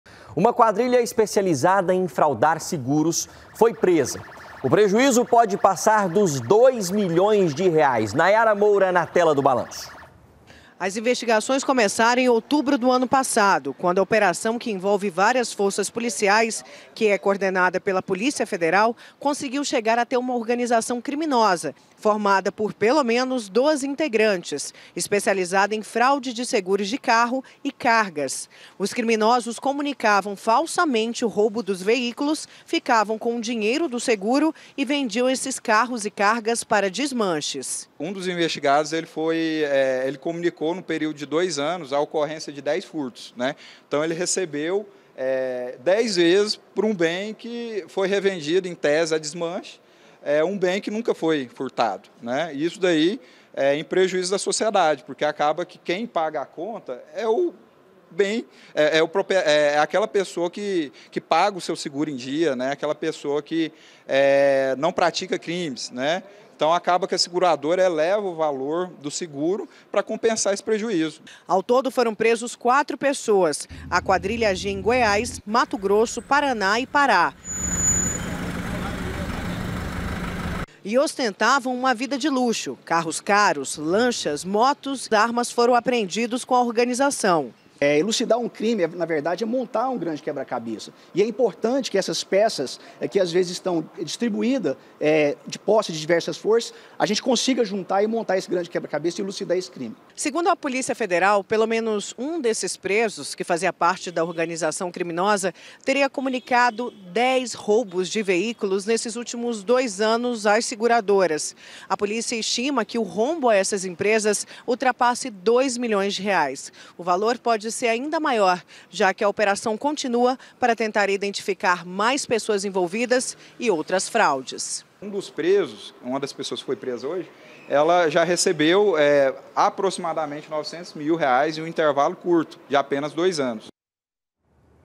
FALSO CRIME: QUADRILHA MENTIA SOBRE ROUBO DE VEÍCULOS
UMA QUADRILHA ESPECIALIZADA EM FRAUDAR SEGUROS FOI PRESA E O PREJUÍZO PODE PASSAR DOS DOIS MILHÕES DE REAIS.